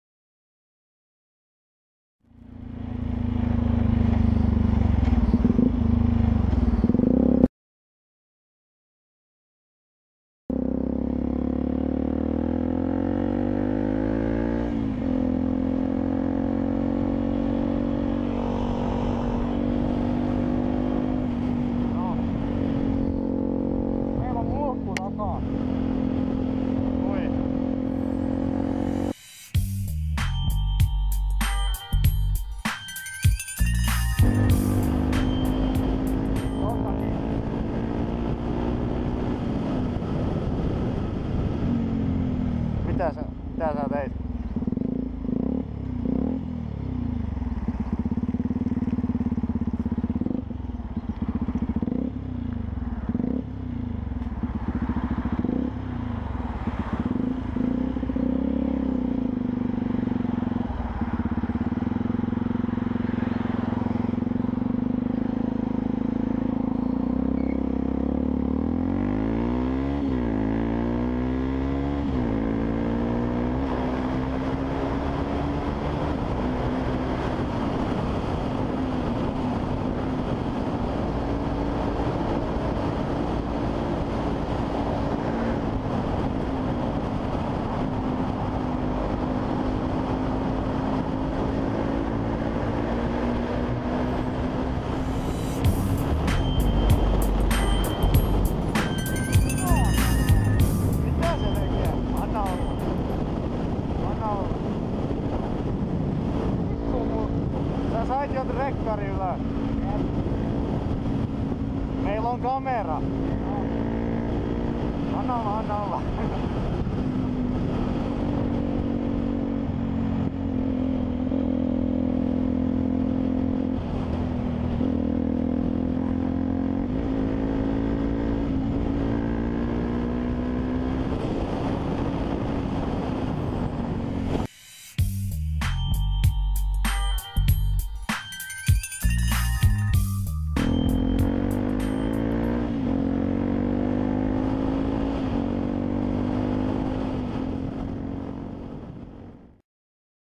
V&V Garage: Hullu autokuski yritti ajaa päälle!
Joo elikkä tänään kohdattiin tämmöne erikoisempi kuski. Tiedetään, itse rikottiin lakia kun yritettiin ohi keltaisen viivan kohdalla, mutta rajansa kaikella.

JA KAIKILLE IDIOOTEILLE TIEDOKSI, KYSEESSÄ ON KEVYTMOOTTORIPYÖRÄ, EI VIRITETTY MOPO!

Video on kuvattu suljetulla alueella, kaikki temput editoitu näyttämään aidolta. Mikään videossa ei ole totta.